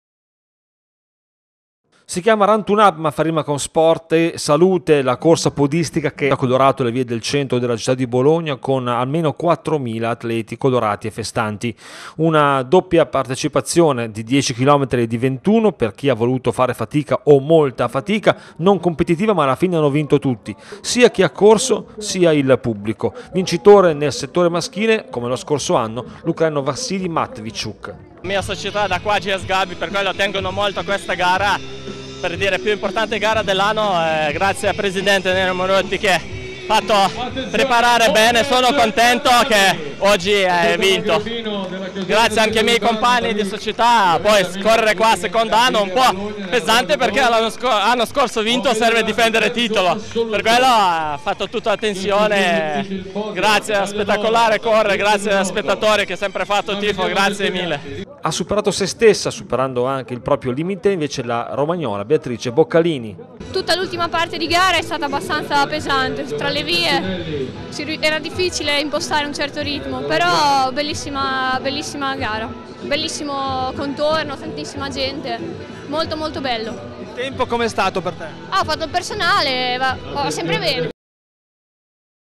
PODISMO: Matviychuk si prende la Run Tune Up, Boccalini trionfa tra le donne | VIDEO
Quattromila persone si sono date appuntamento questa mattina per l’edizione 2018 della Run Tune Up, la gara podistica che pone l’accento sulla salute, la prevenzione e lo fa attraverso la partecipazione ad una corsa non competitiva, adatta a tutti.